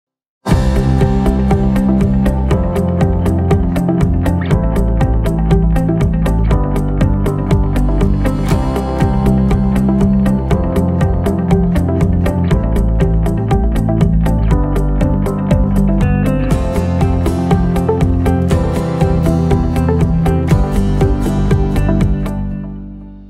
JFCS Ride to Fight Hunger Virtual Ride
Join Jewish Family and Children's Services of Northern New Jersey as we come together to stop hunger in Northern New Jersey one mile at at time. Now more than ever we need your support to provide essential services to our neighbors. Ride or Walk wherever you are.